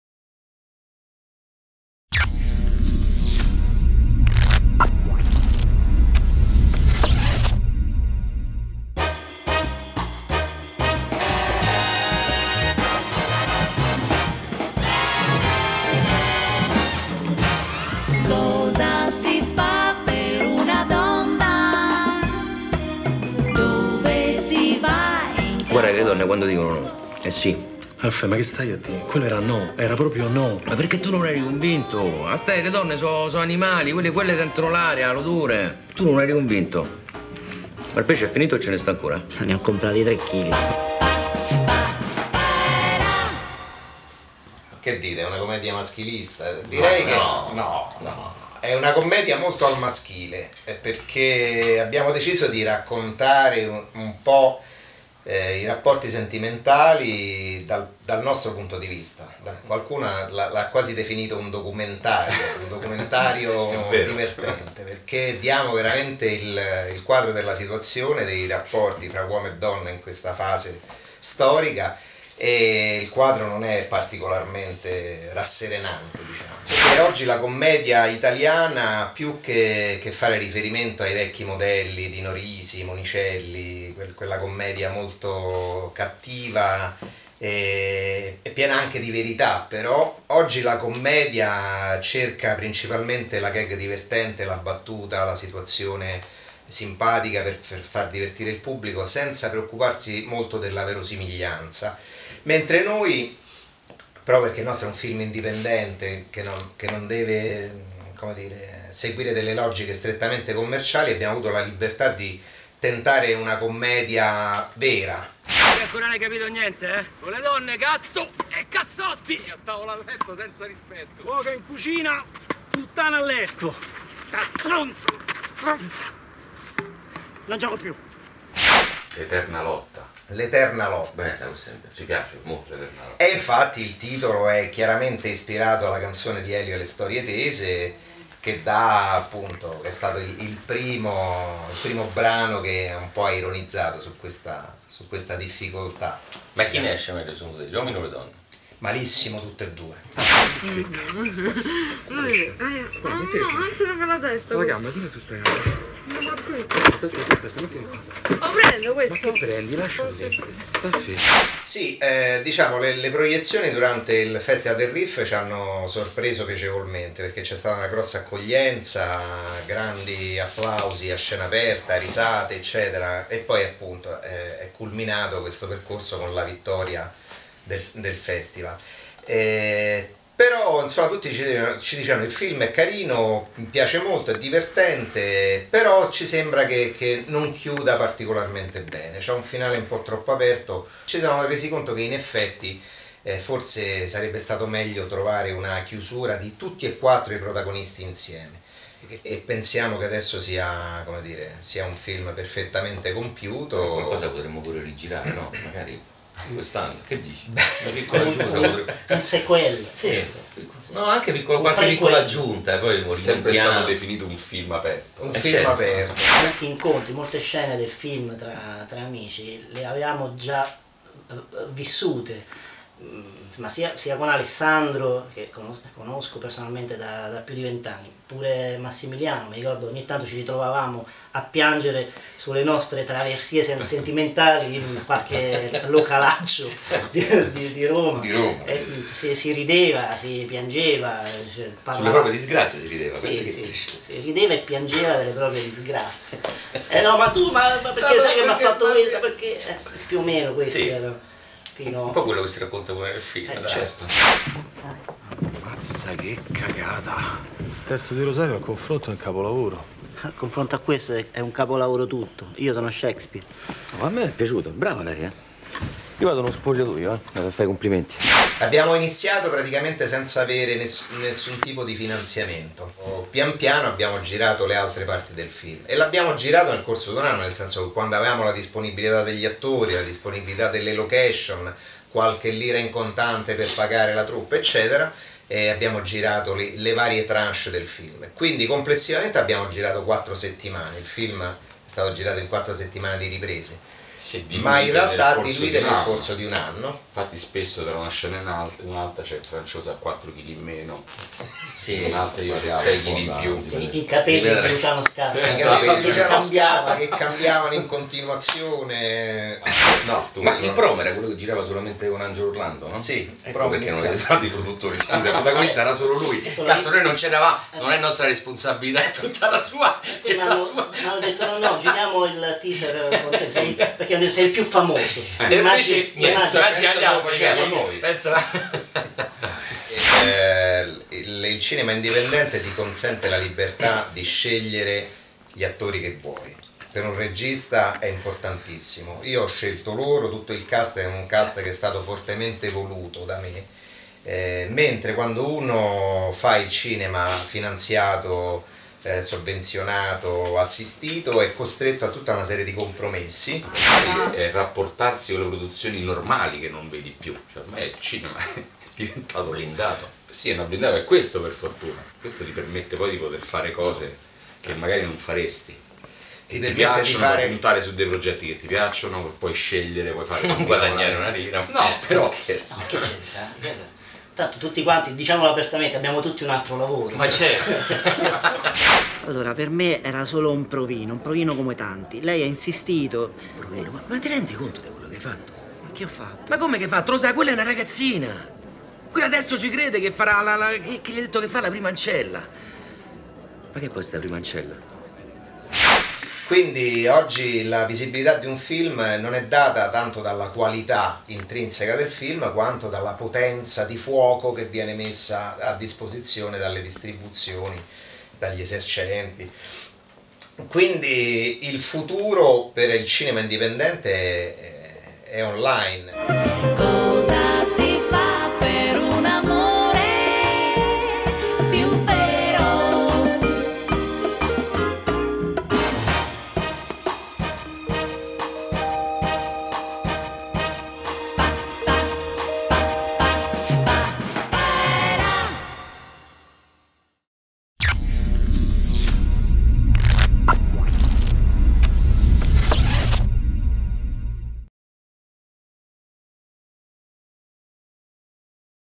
Cara, ti Amo... - Intervista al regista e ai protagonisti
Own Air intervista in esclusiva il regista Gian Paolo Vallati ed i protagonisti del film, Angelo Orlando, Alessandro Procoli, Massimiliano Franciosa.